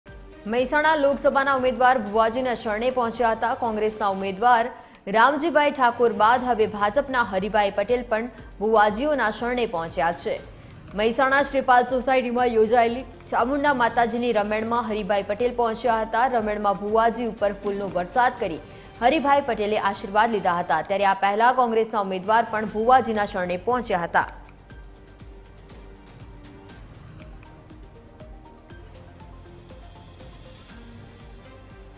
Loksabha Election 2024 : કોંગ્રેસના ઉમેદવાર બાદ હવે ભાજપના હરિભાઈ પટેલ પણ ભુવાજીના શરણે
મહેસાણા લોકસભાના ભાજપ ના ઉમેદવાર પણ ભુવાજીઓના શરણે

કોંગ્રેસના ઉમેદવાર રામજીભાઈ ઠાકોર બાદ હવે ભાજપના હરિભાઈ પટેલ પણ ભુવાજીઓના શરણે

મહેસાણા શ્રીપાલ સોસાયટીમાં યોજાયેલી ચામુંડા માતાજીની રમેણમાં પહોંચ્યા હરિભાઈ પટેલ

રમેણમાં ભુવાજી ઉપર ફૂલ નો વરસાદ કરી હરિભાઈ પટેલે આર્શીવાદ લીધા

આ પહેલા કોંગ્રેસના ઉમેદવાર પણ ભુવાજીઓના શરણે પહોંચ્યા હતા